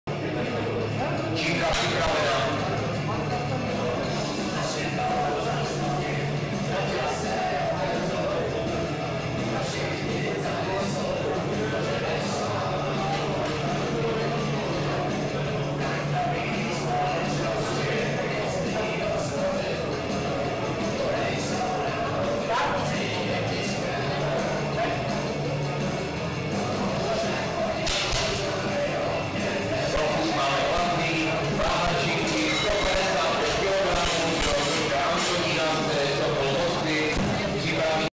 Kornak Adam (1988), BP2, 140 kg
17. ročník Bohumínského benchpressu

Místo: Tělocvična TJ Viktorie Bohumín, Starý Bohumín